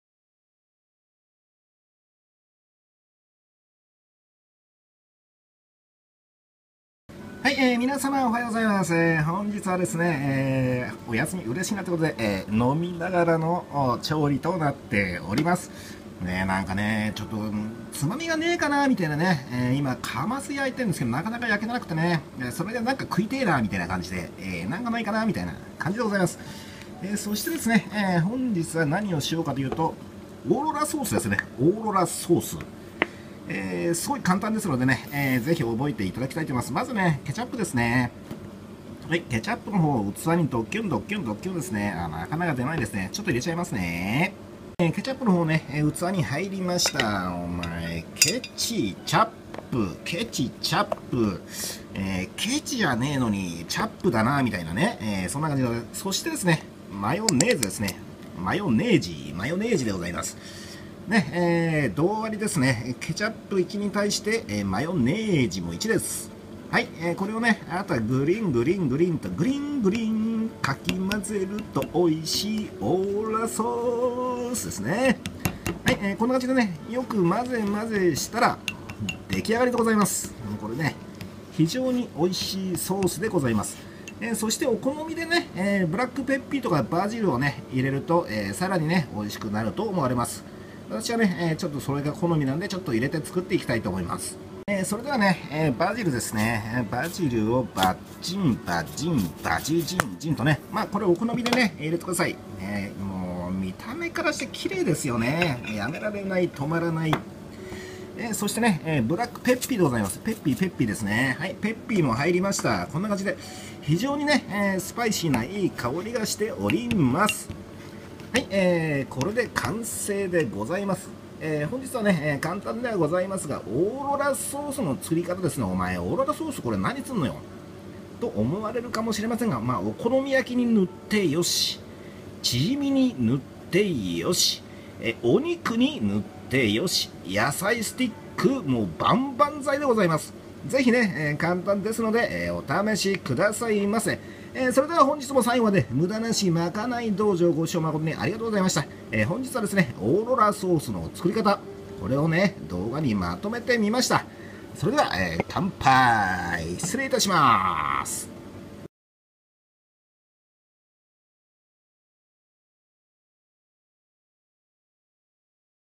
「オーロラソース」作り方
これ一つでサラダ！お魚！お肉料理まですべて網羅！特に野菜スティック、お好み焼きに付けて食べると最高でございます。マヨネーズとケチャップ用意してね(^o^)ｖ「オーロラソース」作り方 ​ @無駄なし！まかない道場（MAKANAI DOUJYOU) ​